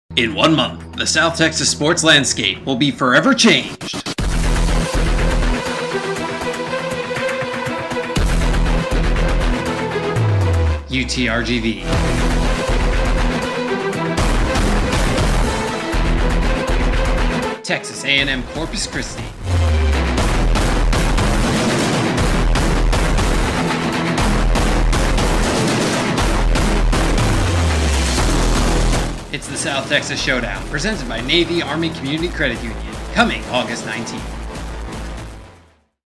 One Month Until The South Texas Showdown, Presented by Navy Army Community Credit Union, Begins
The South Texas Showdown, presented by Navy Army Community Credit Union, is an annual rivalry series that pits the The University of Texas Rio Grande Valley (UTRGV) and Texas A&M University-Corpus Christi (TAMUCC) Departments of Intercollegiate Athletics against each other in baseball, volleyball, women's soccer and golf, and men's and women's basketball, tennis, cross country and outdoor track & field.

The yearly champion is presented with a traveling trophy at the final meeting of the academic year.

The first matchup is in women's soccer on Aug. 19, 2016 at 7 p.m. at UTRGV.

Join the conversation on Twitter by using the hashtag